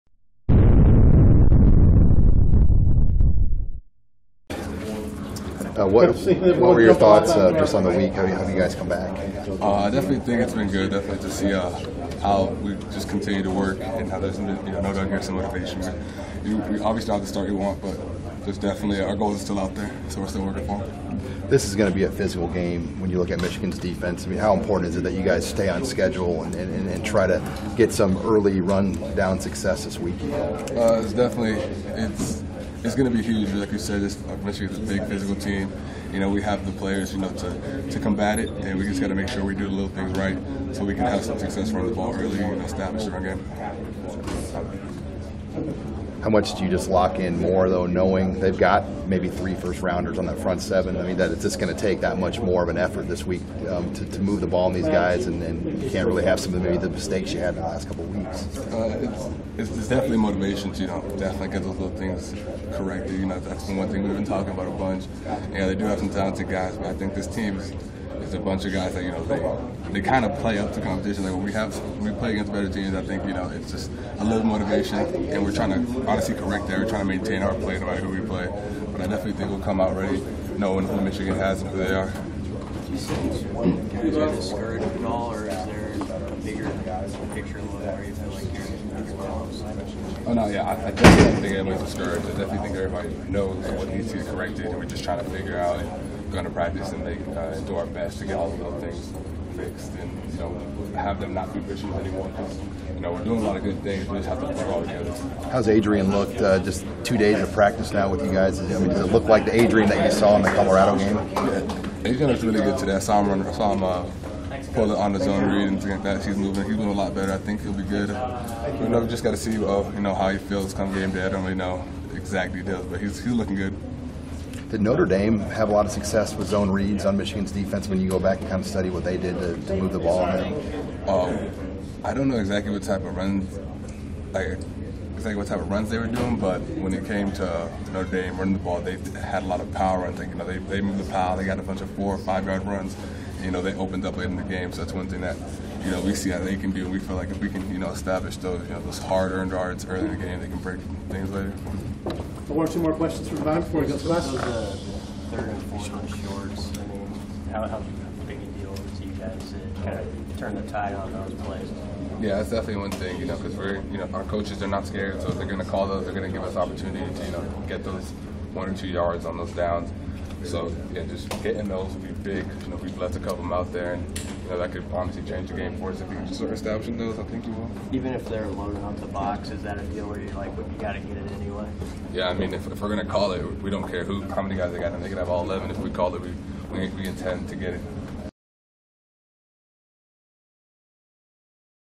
HOL HD: Devine Ozigbo Tuesday Press Conference
Nebraska Running Back Devine Ozigbo meets with the media on Tuesday following practice.